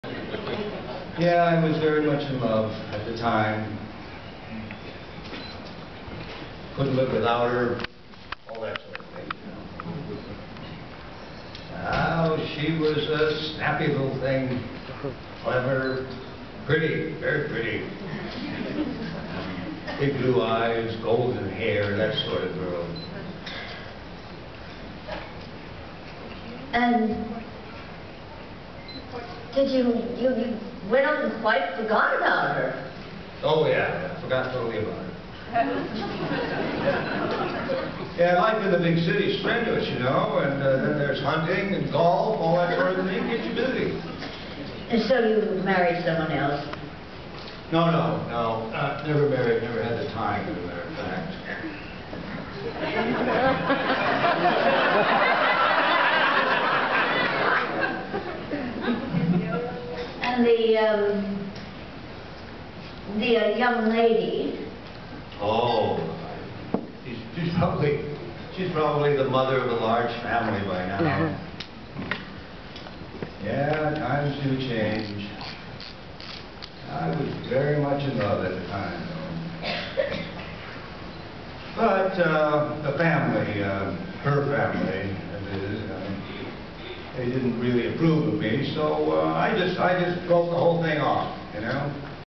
Dark Shadows 2012 Festival Play ~ Pt. 2 ~ Nancy Barrett & Jerry Lacy
Nancy Barrett (Carolyn Stoddard) and Jerry Lacy (Tony Peterson) perform a play together on stage during the Festival Luncheon.